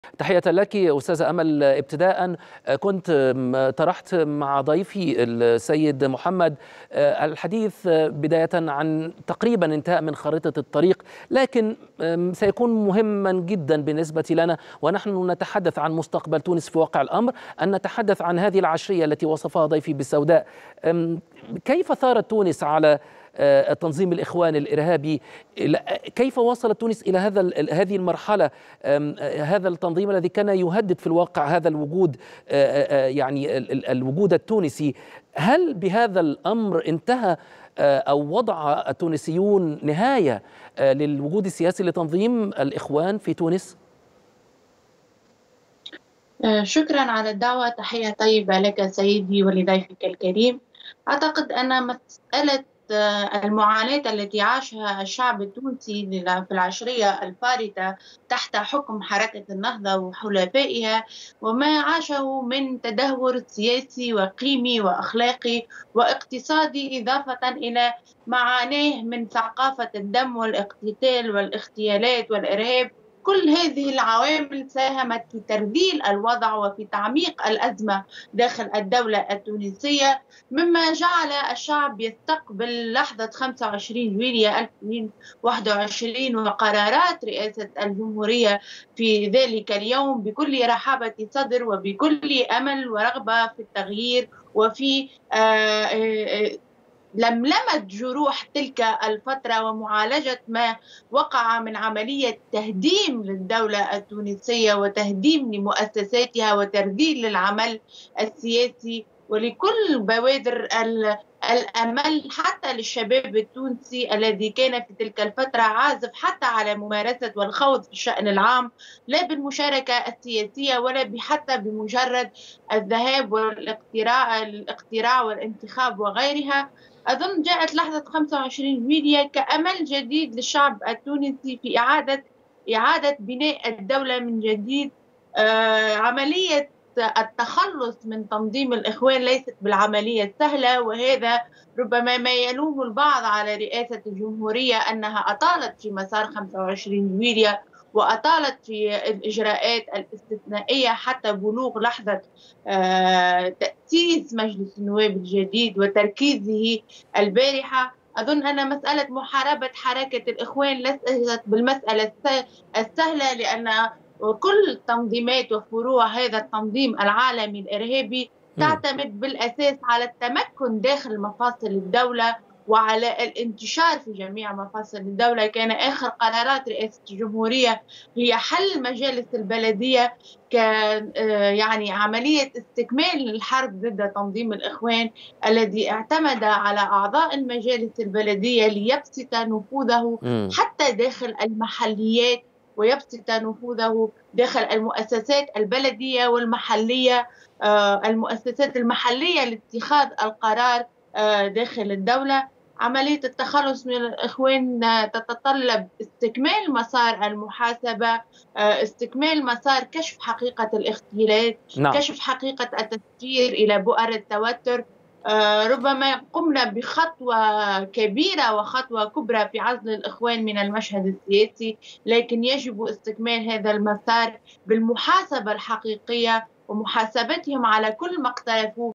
قيادية بحزب التيار الشعبي: عانينا لكي نتخلص من جماعة الإخوان الإرهــ ــابية
أمل الحمروني قيادية بحزب التيار الشعبي: عانينا لكي نتخلص من جماعة الإخوان الإرهــ ــابية

14/3/2023